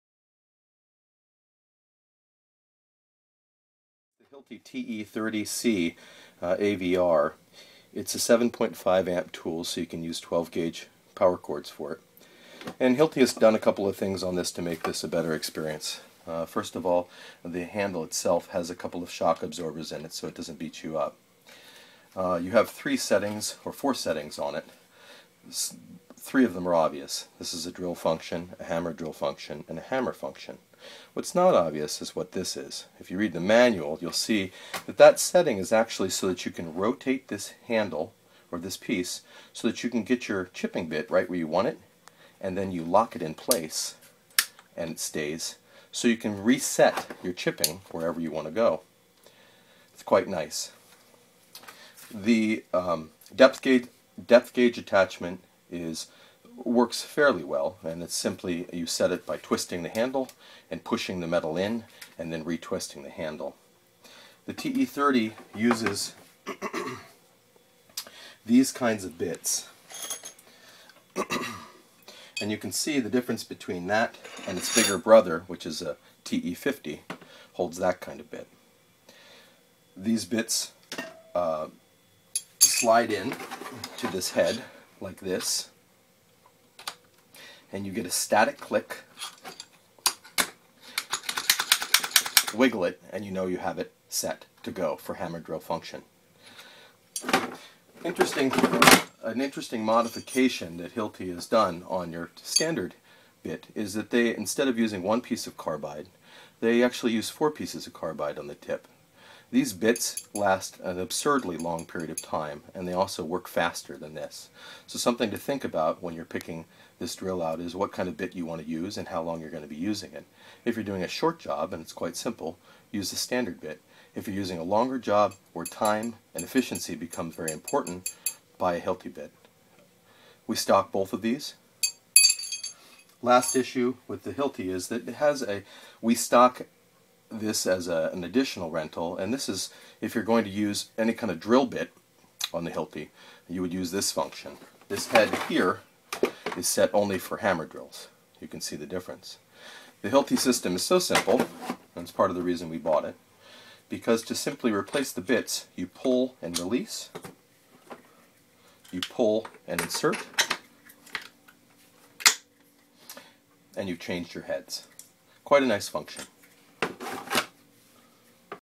Hilti TE 30-C AVR Hammer Drill Discussion
It is rated up to 7.5 amps (850 watts) and produces 750 revolutions  and 4080 impacts a minute.  It has the best power to weight ratio in its class (light hammer drill).  It is for drilling in concrete for rebar, anchors or electrical.   It is also perfect for light, corrective and finish chiseling on concrete and masonry, especially where it is difficult or unnecessary to get a bigger machine involved.

The Hilti TE 30 is powered to hammer drill 1/4" holes to 1" (see manual for other uses) holes in concrete all day long.  If you need more power, look at the TE-50.  Let us know if this helps.This video discussion was created